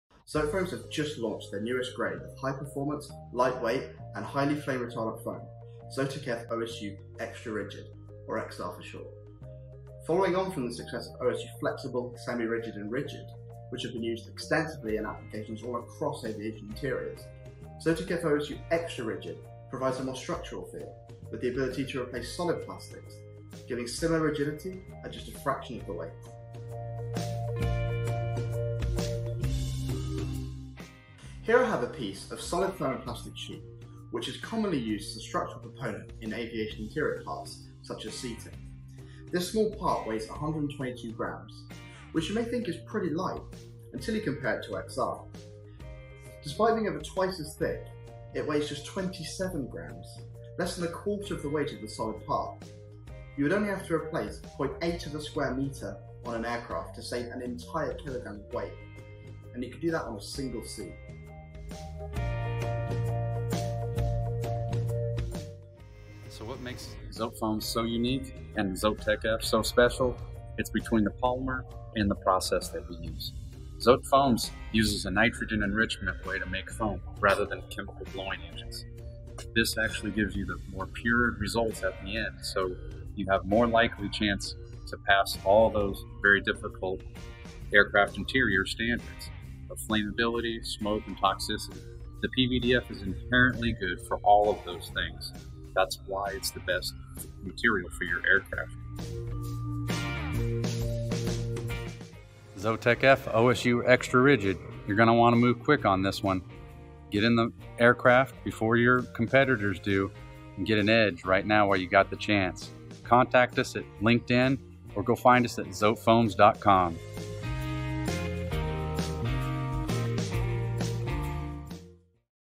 Introducing ZOTEK® F XR high-performance foam for aircraft interiors
ZOTEK® F XR is Zotefoams' latest disruptive material for aircraft interiors. In this video, our product specialists explain the suite of benefits this lightweight, semi-structural high-performance foam offers to aircraft manufacturers and operators, including weight savings of up to 70% compared to traditional materials, such as solid thermoplastics and silicons.